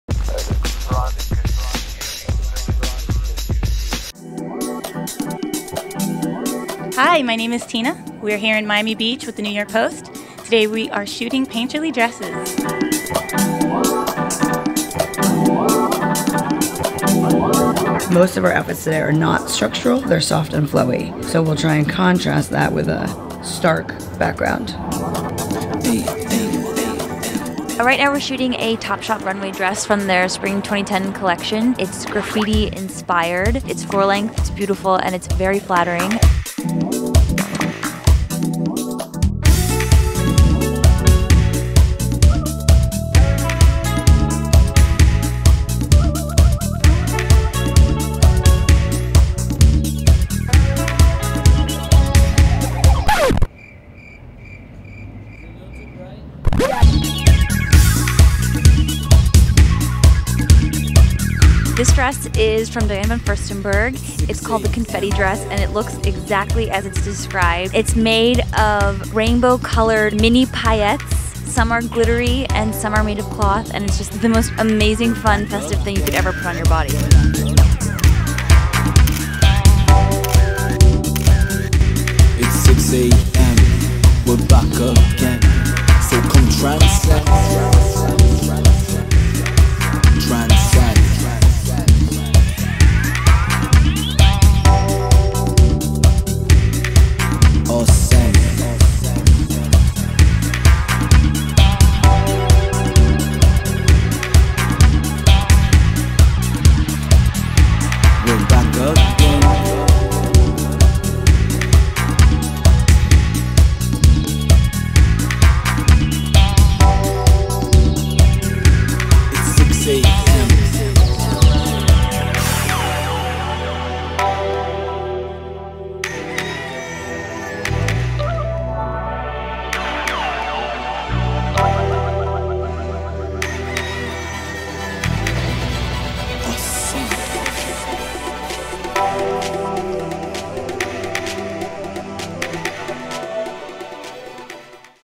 Paint The Town - New York Post
Beauty unfolds behind the scenes of a Pulse fashion shoot in the art district of South Beach, Miami.